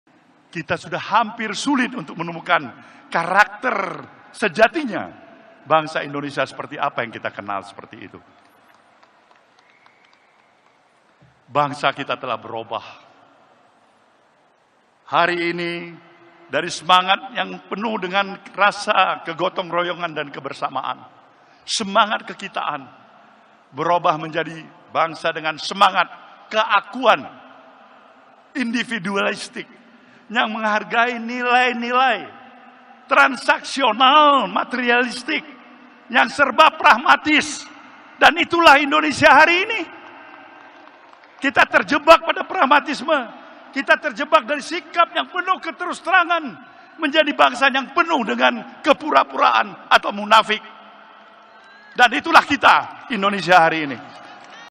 SURYA PALOH Sebut Indonesia Kini Menjadi Bangsa Munafik
TRIBUN-MEDAN.COM - Ketum NasDem Surya Paloh menyebutkan kini Indonesia berubah menjadi bangsa yang munafik kepada ribuan kader NasDem di GBK, 16 Juli 2023.